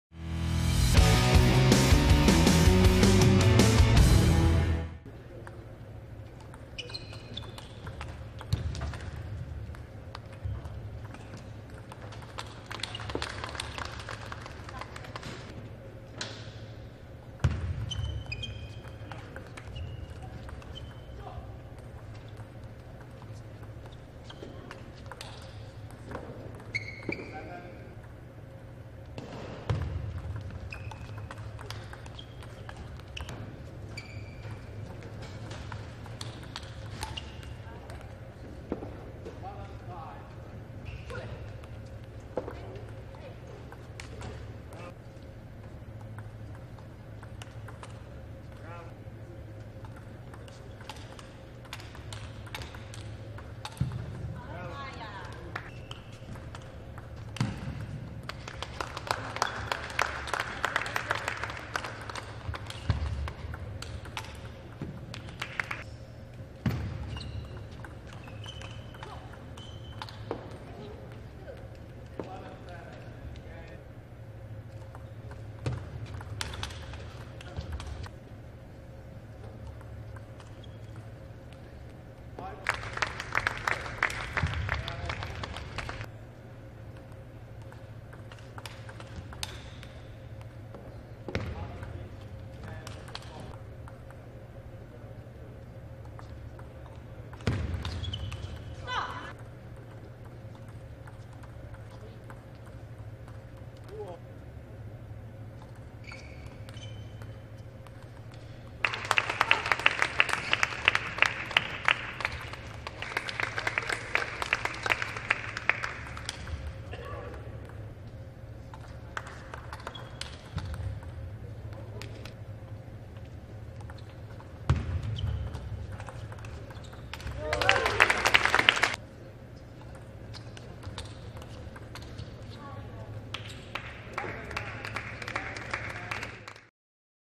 2016 Women’s World Cup Highlights: Georgina Pota vs Zhenhua Dederko (Qual)
Review all the highlights from the Georgina Pota vs Zhenhua Dederko (Qual) match from the 2016 Women’s World Cup